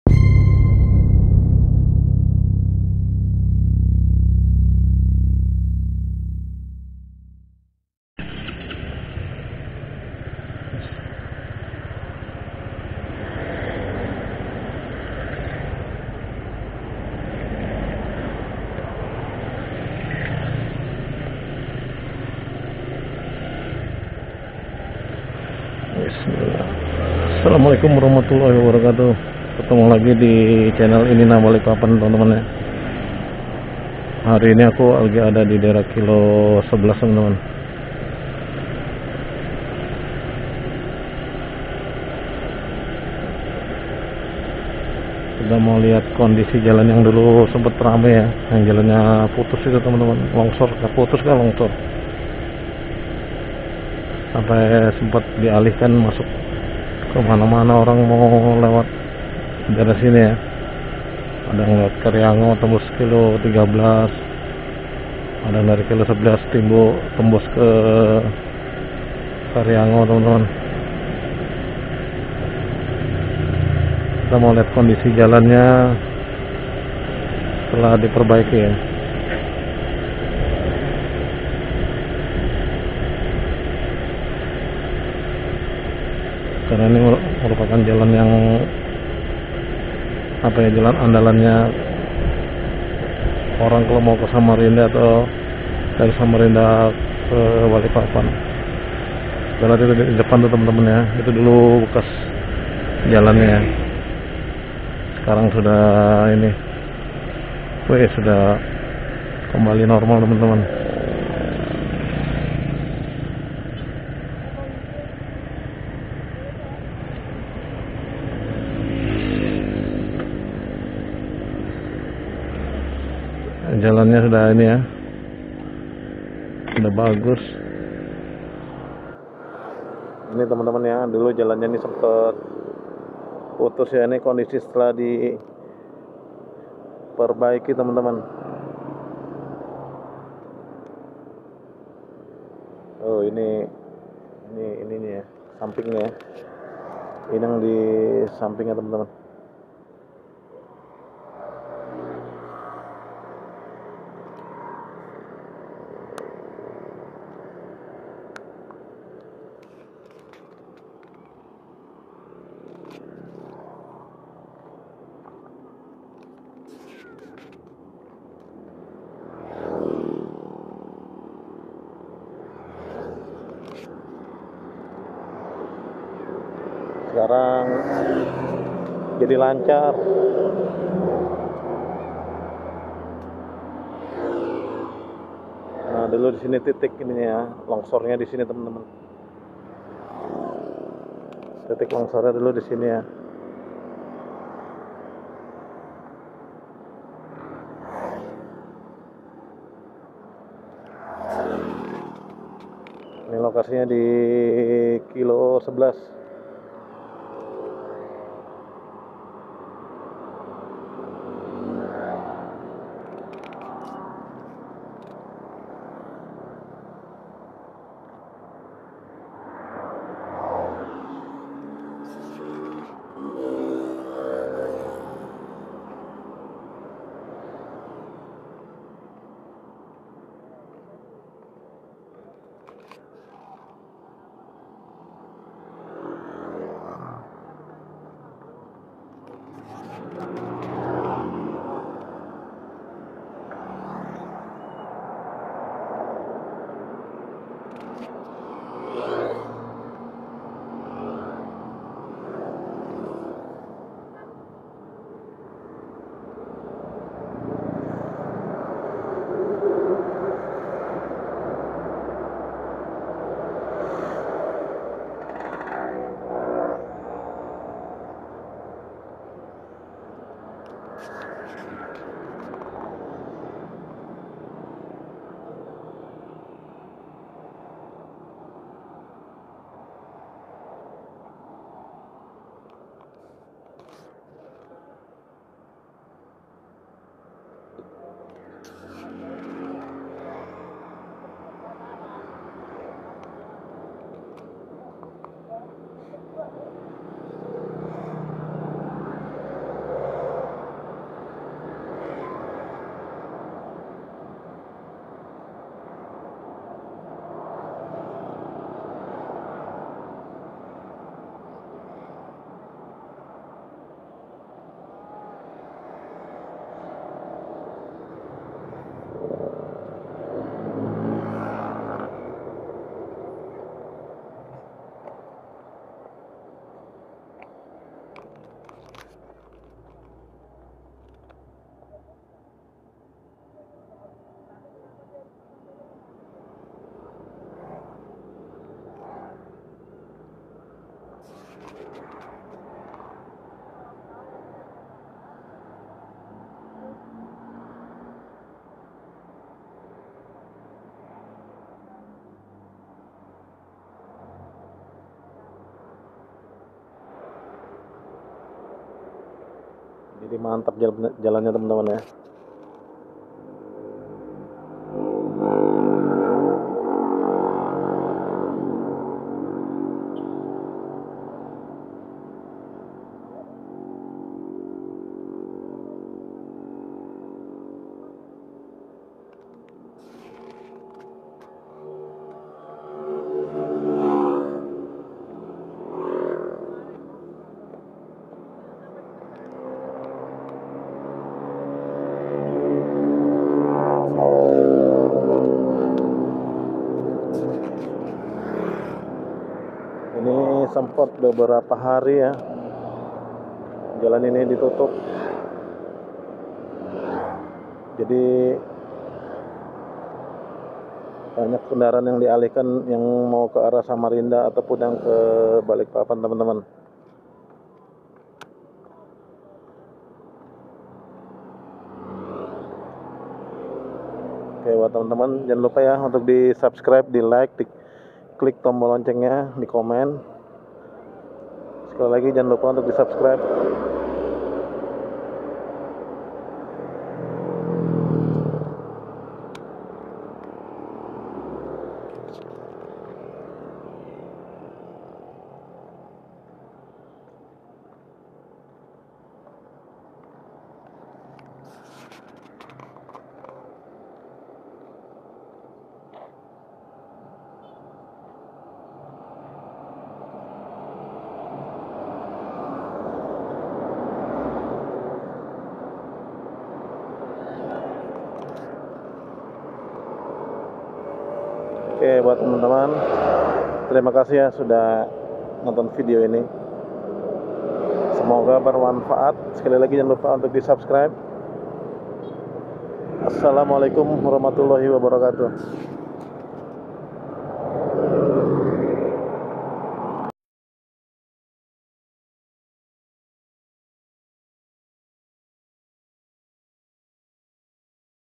JALAN KILO 11 BALIKPAPAN
Sempat amblas parah,akibatnya jalannya di alihkan sekarang jalannya sudah bagus, kilo 11 kini sudah ndak macet lagi,.
videoBalikpapanhariini videoviral Balikpapannews jalansoekarno-hattaBalikpapan